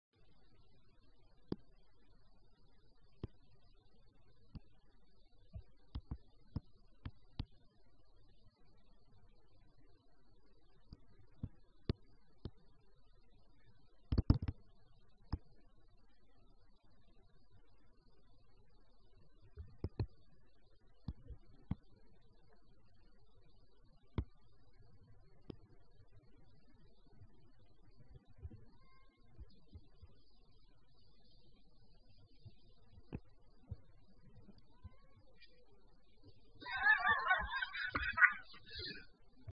Cat leap fail 😂
Poor kitty thought he could make the leap